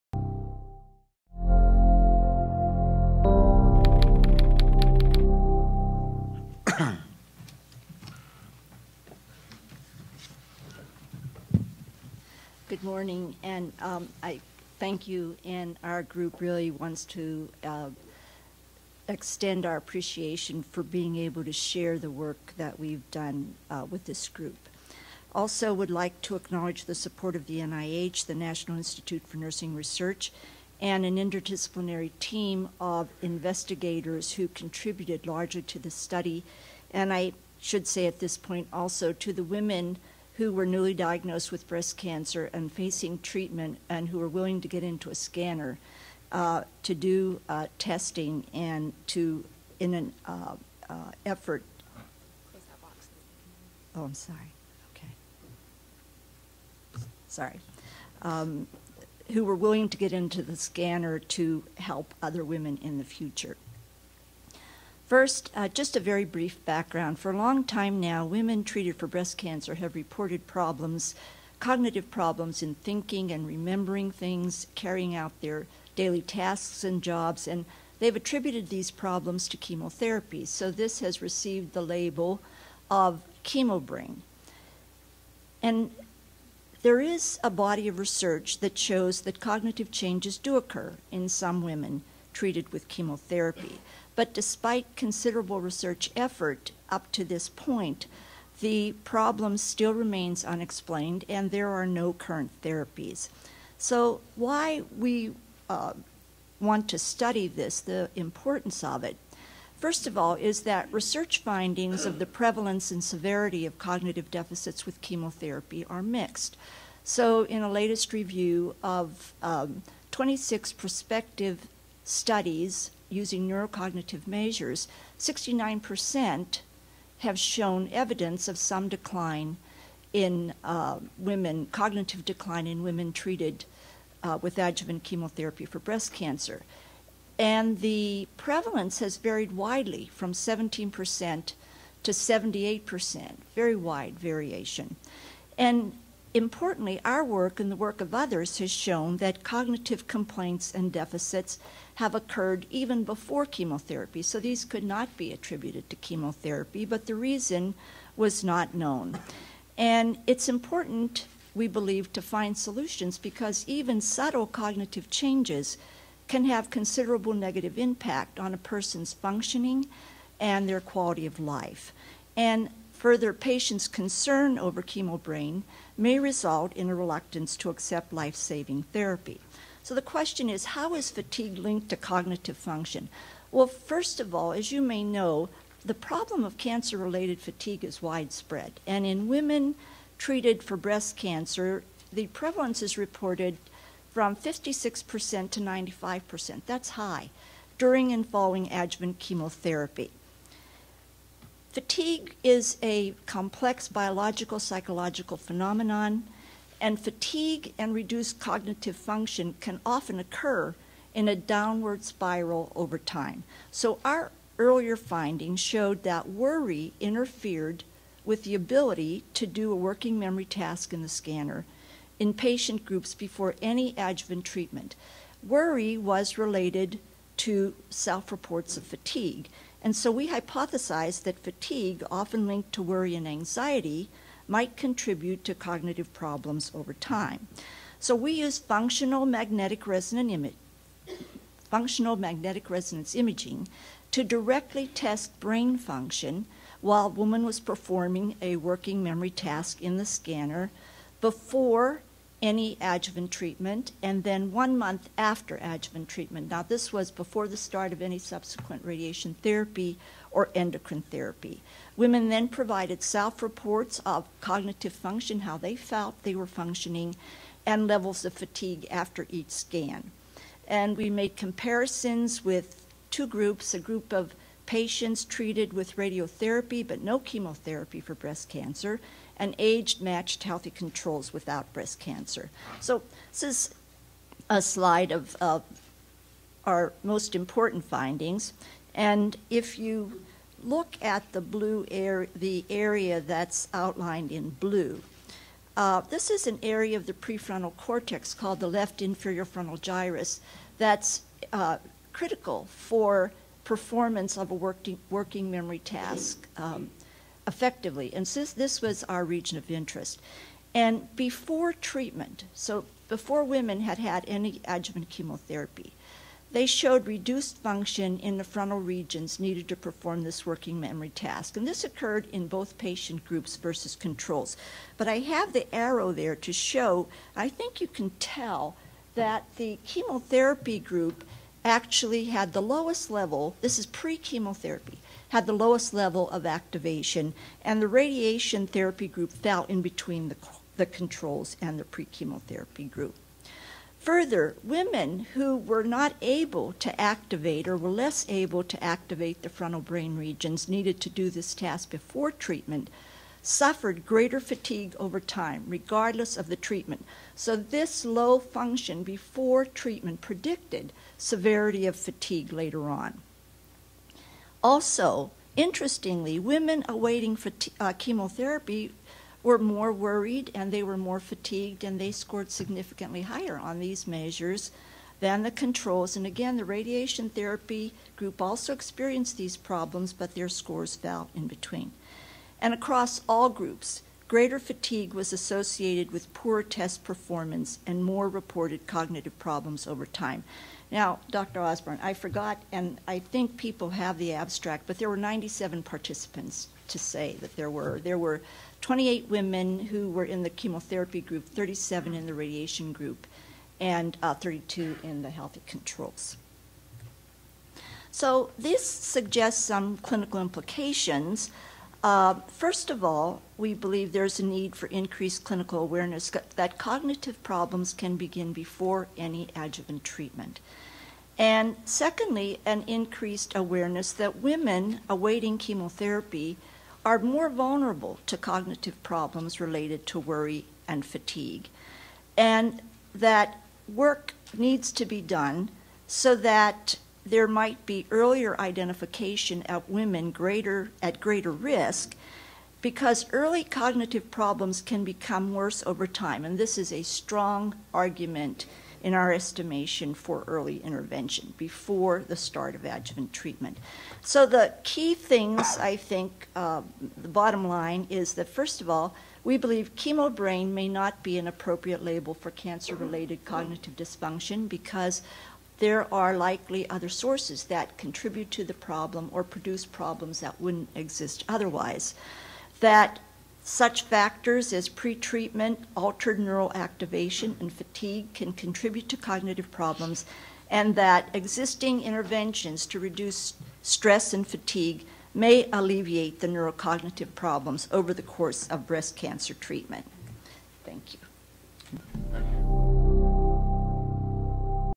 Bernadine Cimprich, Nursing, University of Michigan, Ann Arbor, MI, USA
Bernadine Cimprich, Nursing, University of Michigan, Ann Arbor, MI, USA: Neurocognitive impact in adjuvant chemotherapy for breast cancer linked to fatigue: A Prospective functional MRI study.
Press Conference 3 San Antonio Breast Cancer Symposium 2012, Friday December 7, 2012.